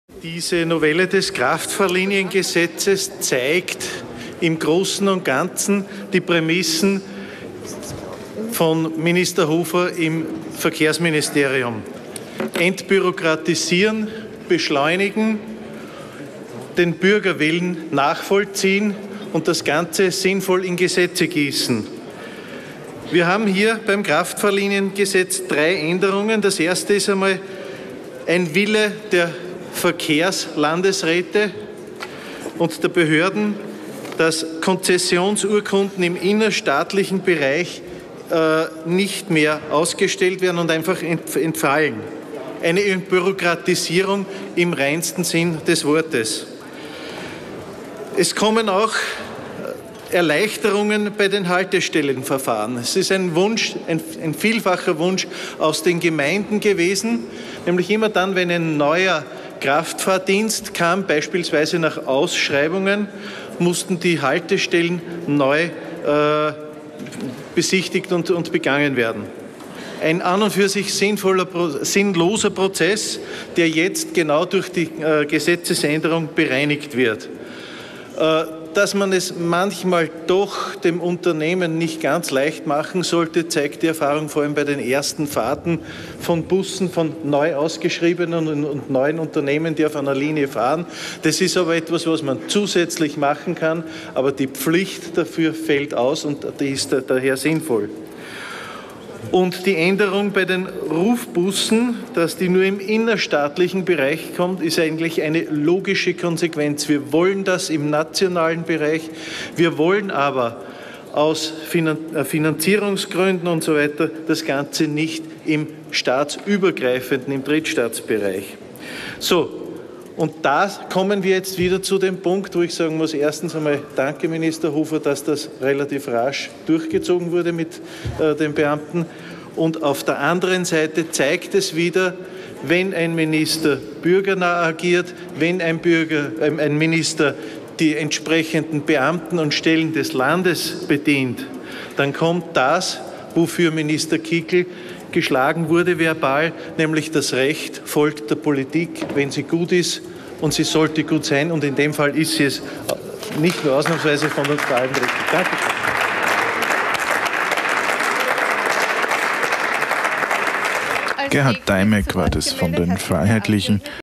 Gerhard Deimek - Kraftfahrliniengesetz - 30.1.2019
FPÖ-Abgeordneter Dipl.-Ing. Gerhard Deimek im Zuge der Debatte über das Kraftfahrliniengesetz am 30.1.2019.